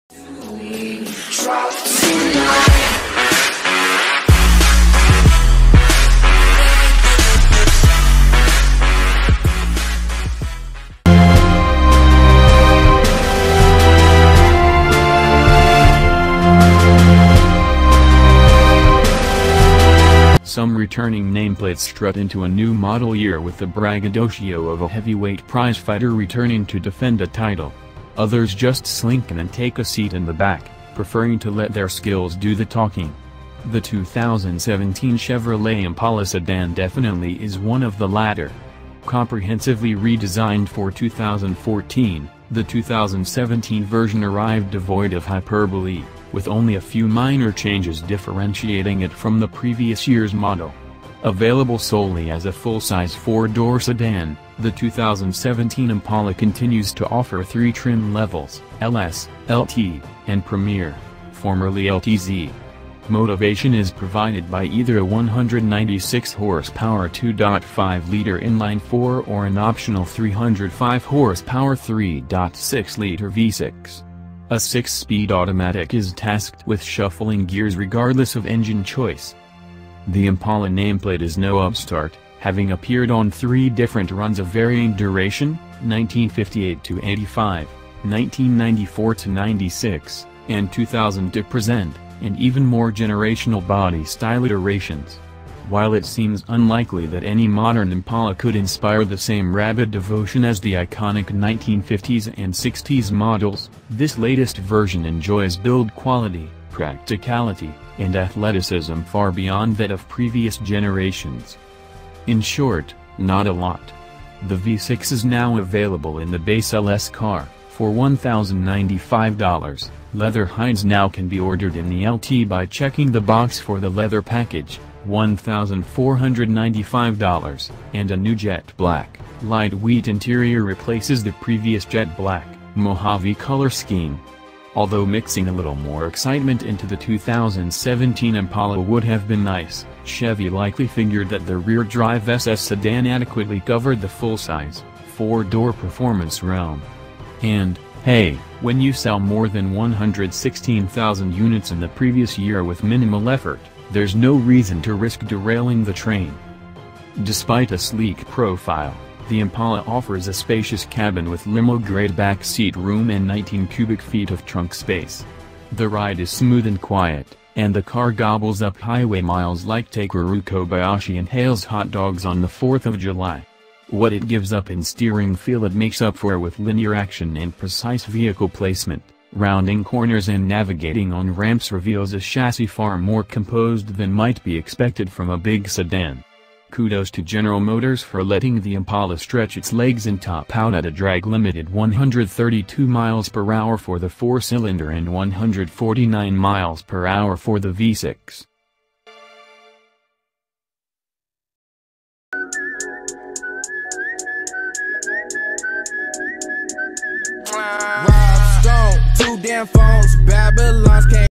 [Wacth] News 2017 Chevrolet Impala Review | Auto Today
[Wacth] News 2017 Chevrolet Impala Review. In short, not a lot. The V-6 is now available in the base LS car (for $1095), leather hides now can be ordered in the LT by checking the box for the Leather package ($1495), and a new Jet Black/Light Wheat interior replaces the previous Jet Black/Mojave color scheme. Although mixing a little more excitement into the 2017 Impala would have been nice, Chevy likely figured that the rear-drive SS sedan adequately covered the full-size, four-door performance realm. And, hey, when you sell more than 116,000 units in the previous year with minimal effort, there’s no reason to risk derailing the train.